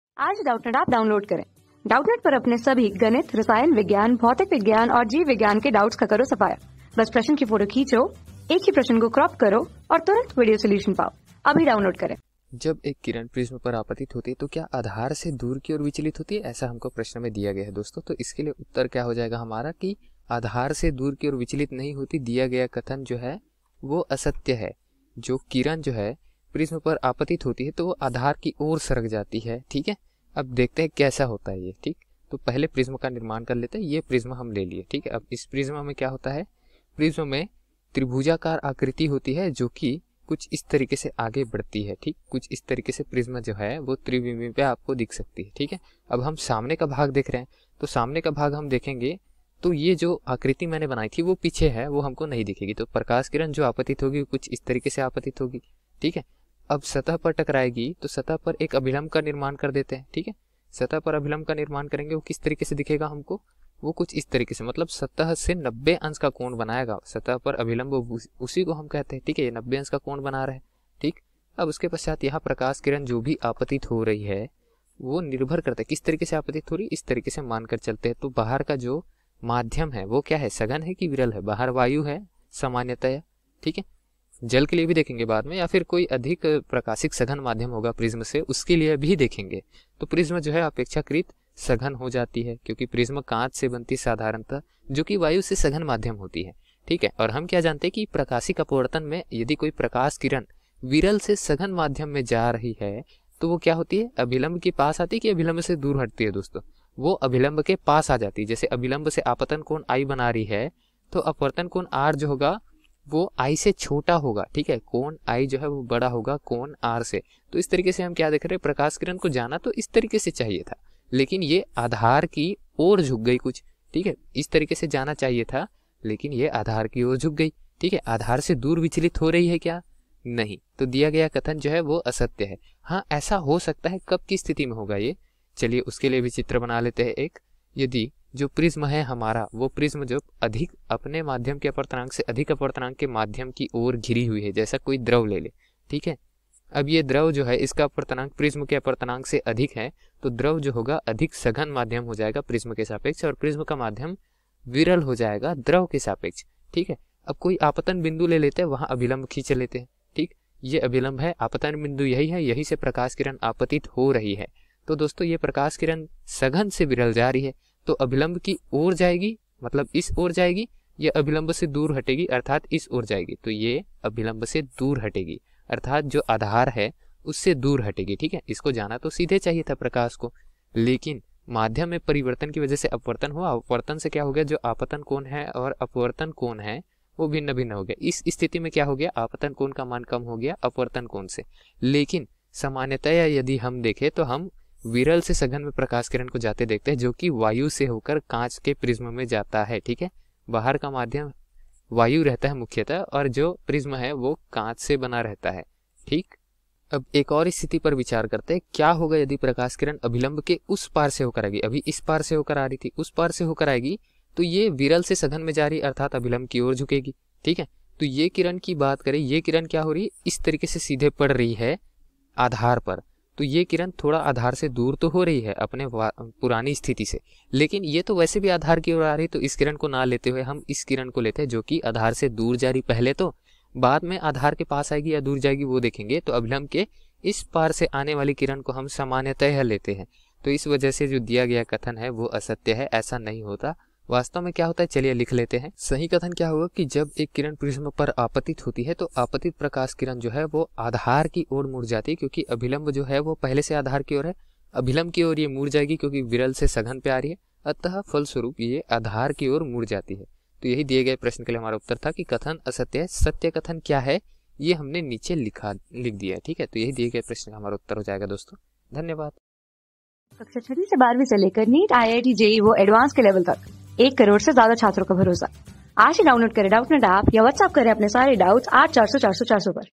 जब एक किरण प्रिज्म पर आपतित होती है तो क्या आधार से दूर की ओर विचलित होती है ? | 12 | किरण प्रका...
जब एक किरण प्रिज्म पर आपतित होती है तो क्या आधार से दूर की ओर विचलित होती है ?
Class: 12
Subject: PHYSICS
Chapter: किरण प्रकाशिकी एवं प्रकाशिक यन्त्र
Board:IIT JEE

👉 Have Any Query? Ask Us.